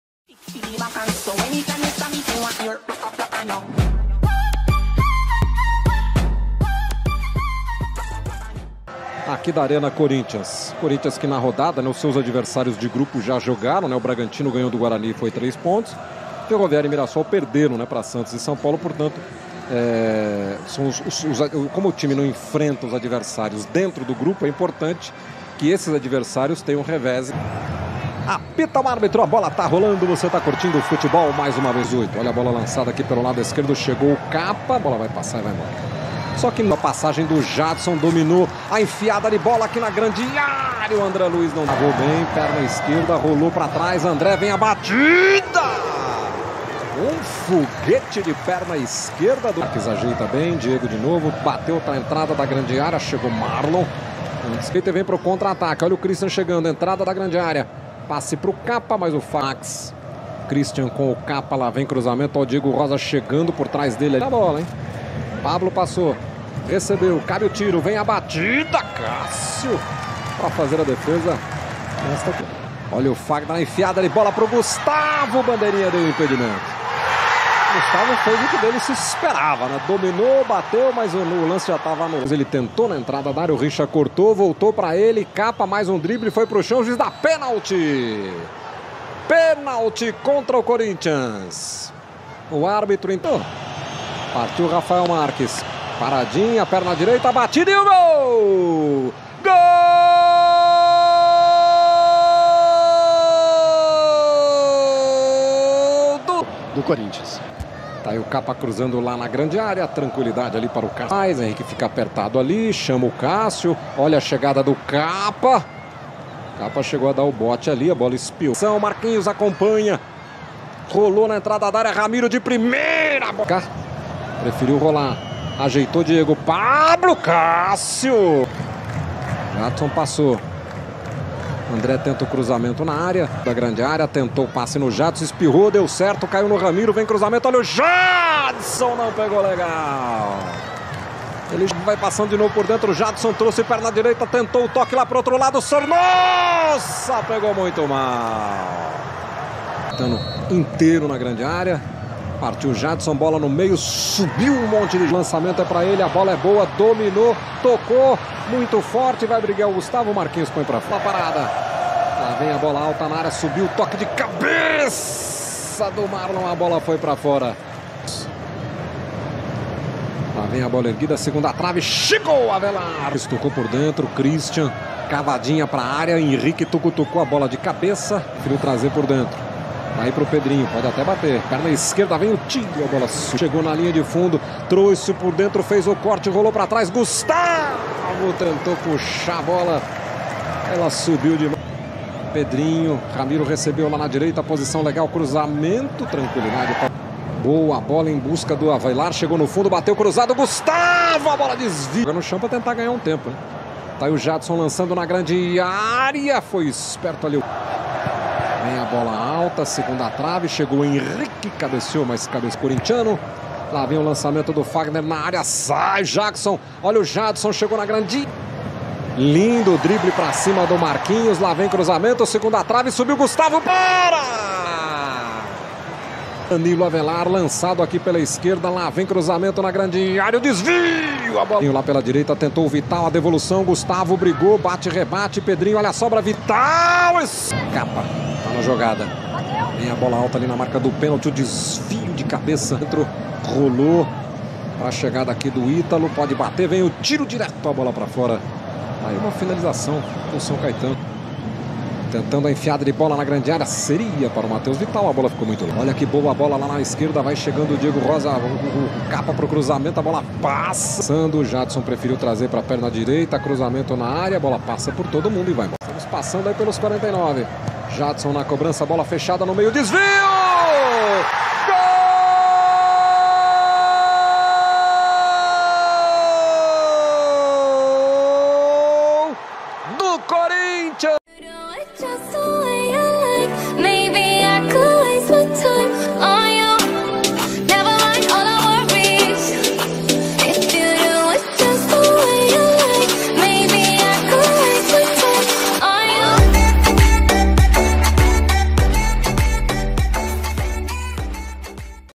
Corinthians 1 x 1 São Caetano - Melhores Momentos e Gols - PAULISTÃO 20/01/2019
A estreia do Corinthians na temporada de 2019 não saiu como o esperado neste domingo (20), mas o empate no último minuto diminuiu a frustração na Arena Corinthians. O São Caetano saiu na frente com o ex-palmeirense Rafael Marques e até segurou bem o resultado, mas Henrique garantiu o 1 a 1 no último lance da estreia dos times no Campeonato Paulista.